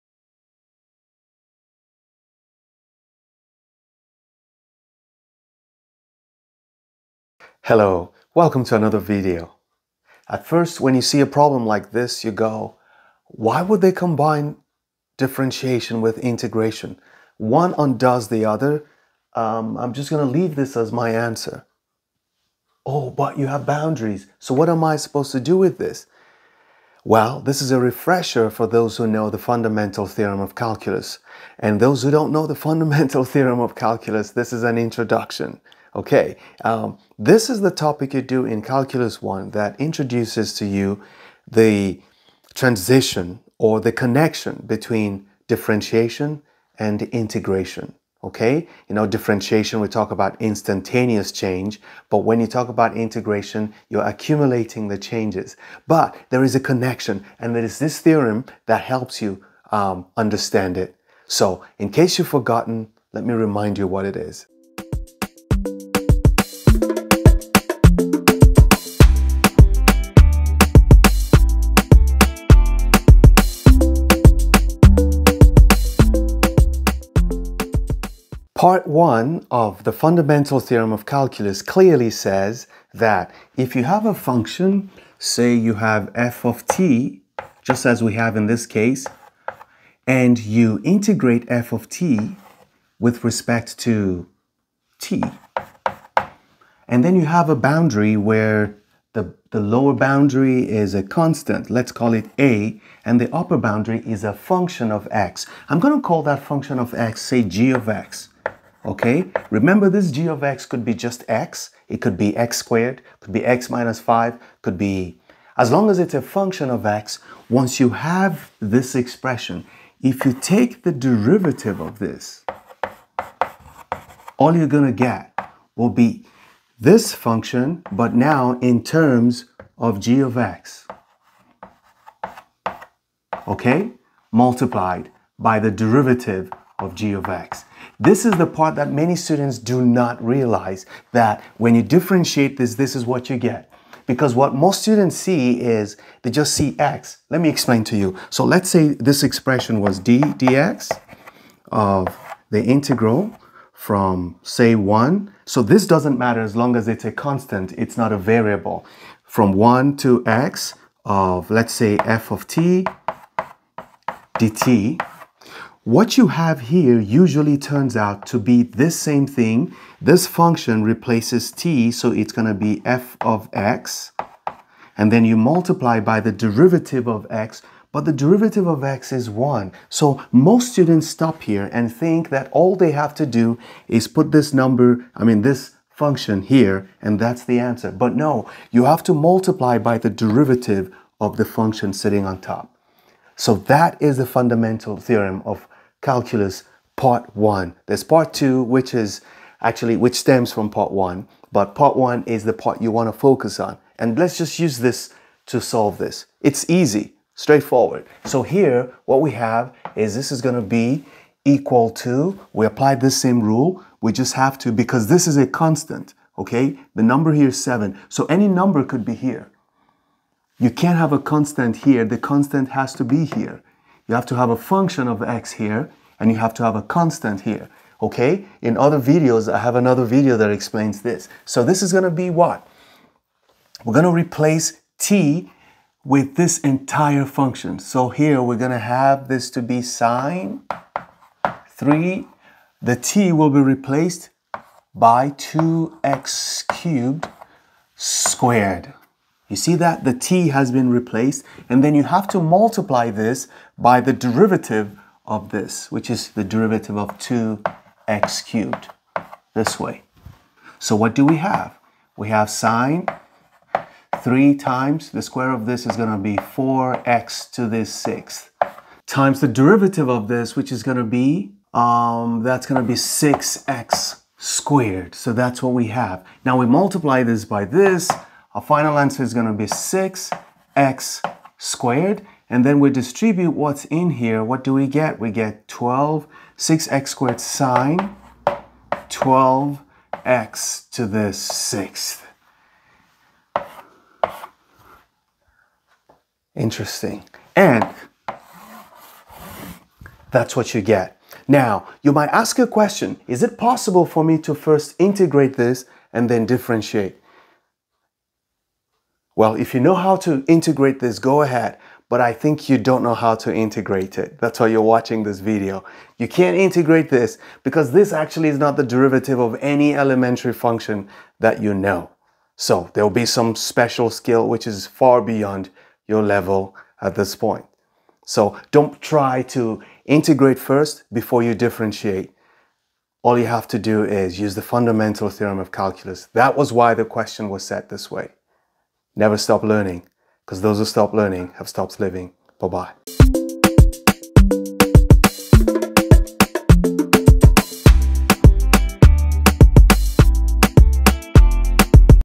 Fundamental Theorem of Calculus - Part I
In this video, I showed how to use the FTC part 1 to evalutae the derivative of an integral  function.
 Link to previous video mentioned